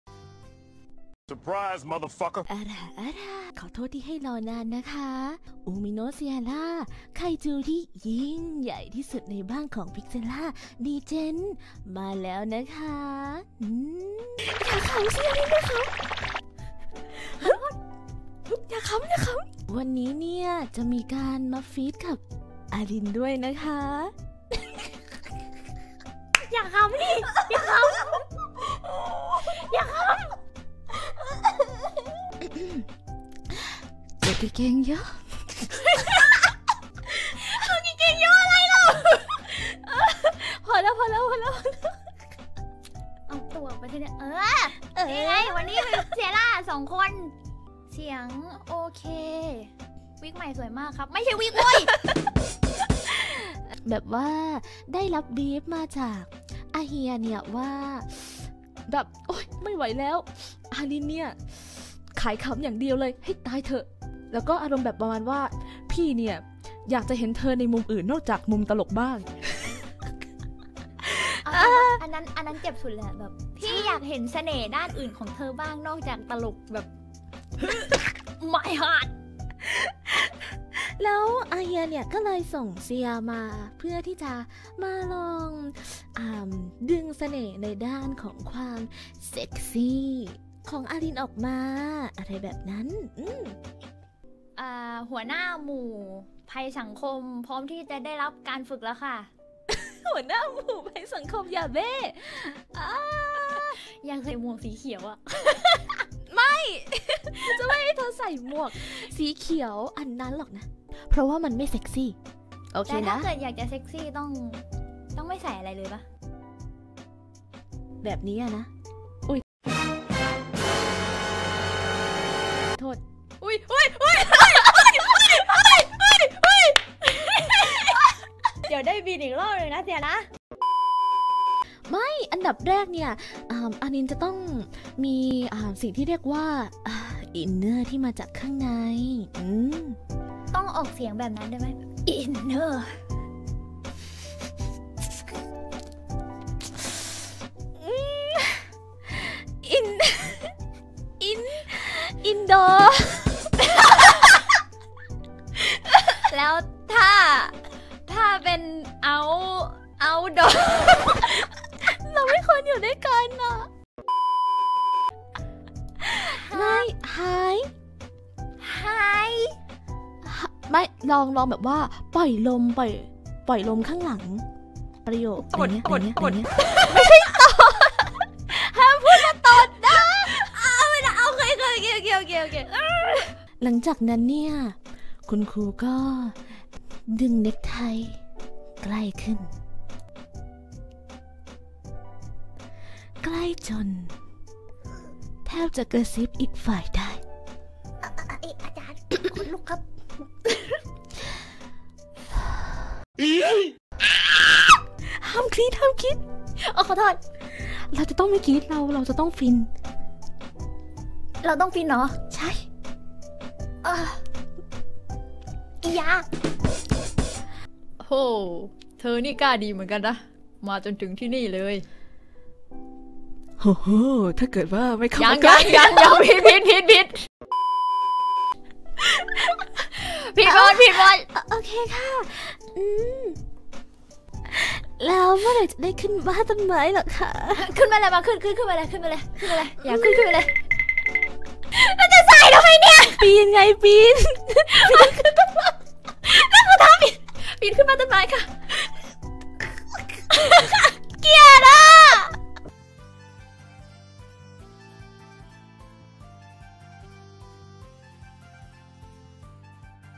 [Vtuber]Hi-light เซียร่า สอน อาลิน ให้ SEXY
ขาย Sexy(ไม่) - ขายขำ (ใช่) หัวจะปวด

ตามไปตำและดูไลฟ์ย้อนหลังได้ที่นี่